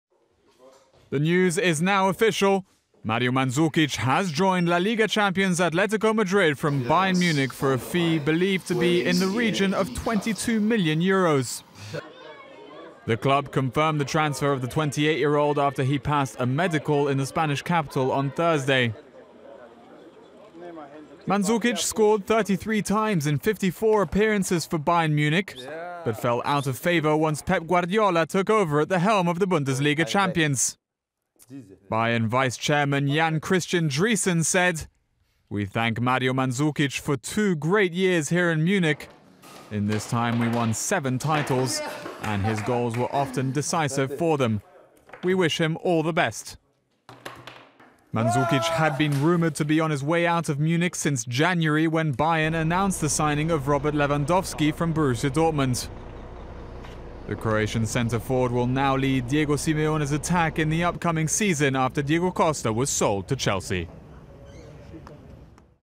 Mandzukic signs for Atletico Madrid
Croatia striker Mario Mandzukic has joined Spanish Premier League champions Atletico Madrid from Bundesliga title holders Bayern Munich - the club confirmed on Friday that the 28-year-old had completed the transfer deal worth an estimated will cost around 22 million euros (29 million US dollars) after passing a medical in the Spanish capital city.